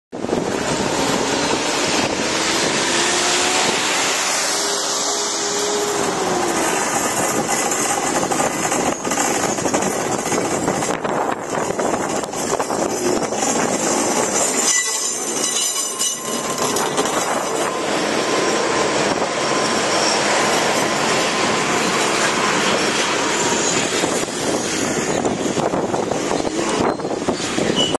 ＤＬ－１０６帰庫
「紀勢貨物」２０８８レに貨車を送り込み、紀州製紙専用線国道４２号踏切を通
過する、ＤＬ－１０６の様子。